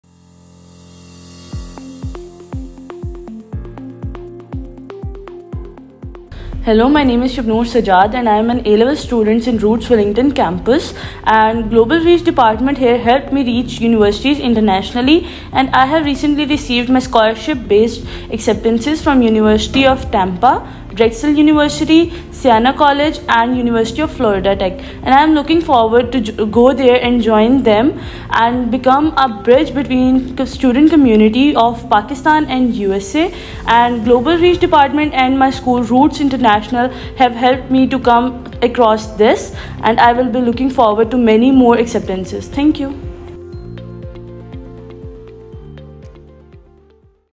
Shabnoor Sajjad her Incredible University Acceptances: Unveiling a Journey of Academic Triumph!
We are proud to announce our students' acceptances into multiple high profile international educational institutes from around the world. It is testament to RISC's commitment to top notch, second to none education standards